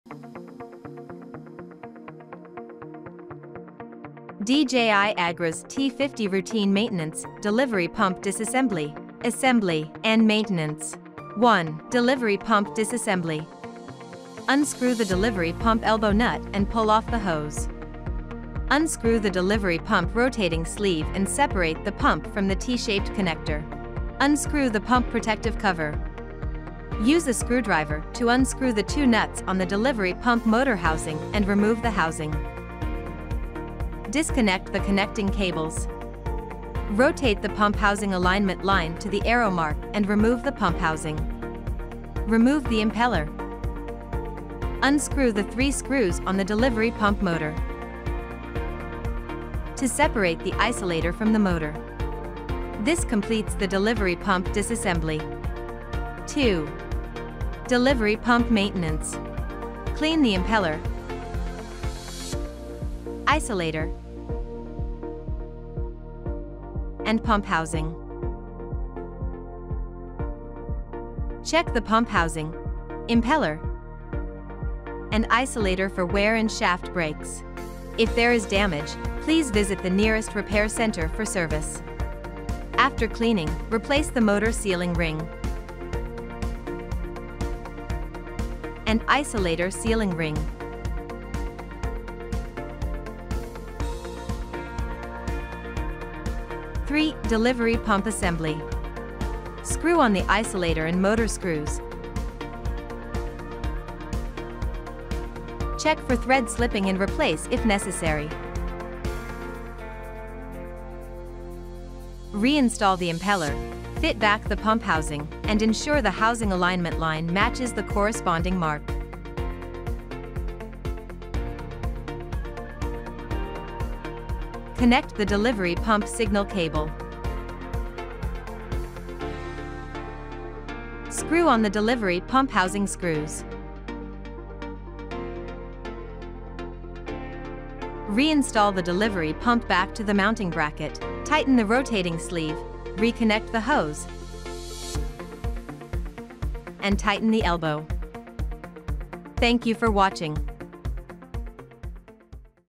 Delivery Pump Maintenance | Agras Tutorial T50&25
Ensure your delivery pump operates at its best with our comprehensive maintenance tips, helping you avoid downtime and keep your operations flowing seamlessly!